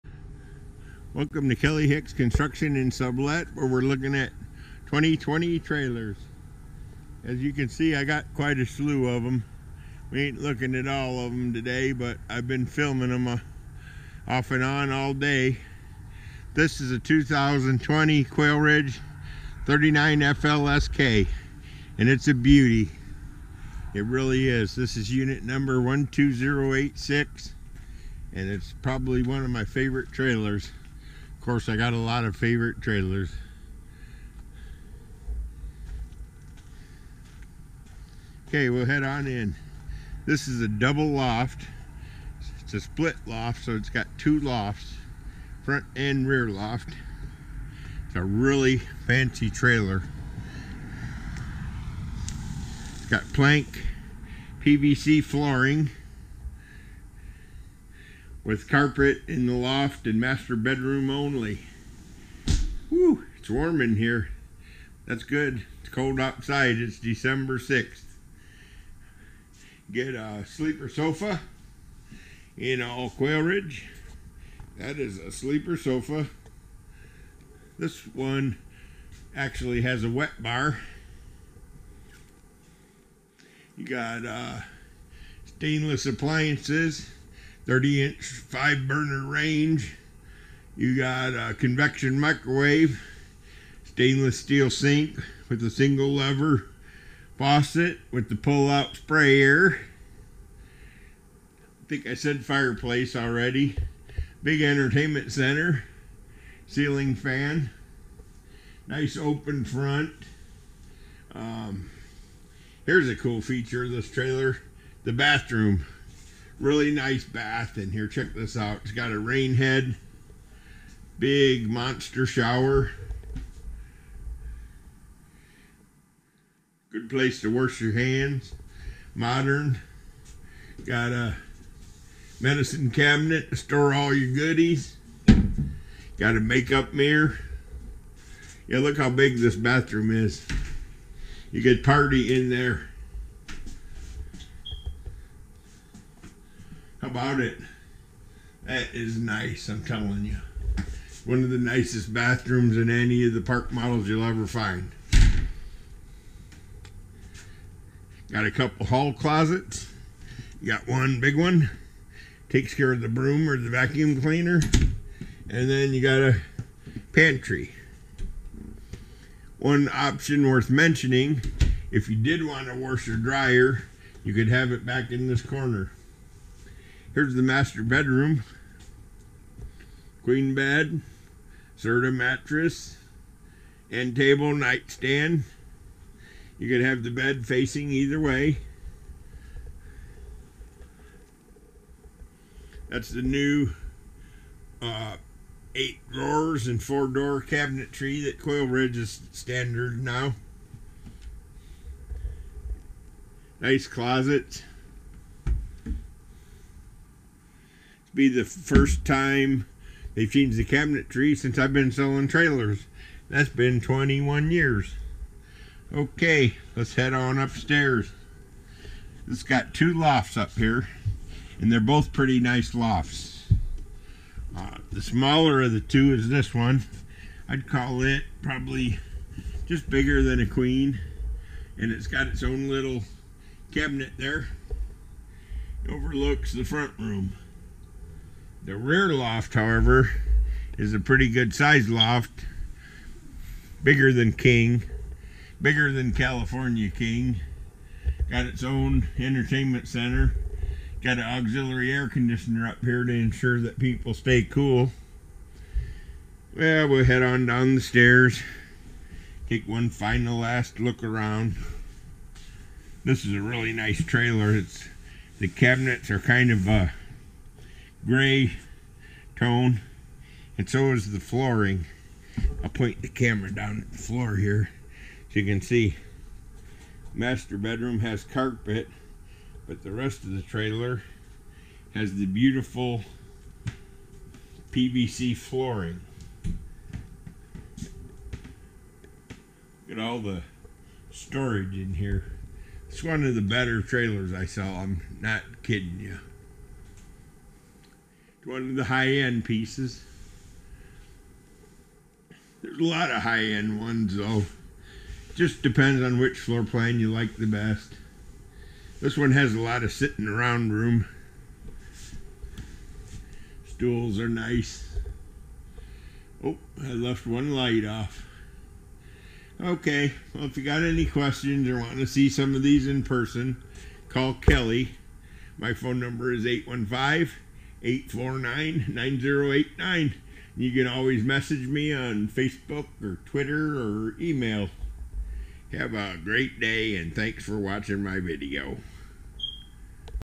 New Quailridge 39FLSK Luxury Park Model RV by Forest River Inc (Tiny house on Steroids)
One of the best Park Model Floorplans out there! Video courtesy of Kelly Hicks RV Sales.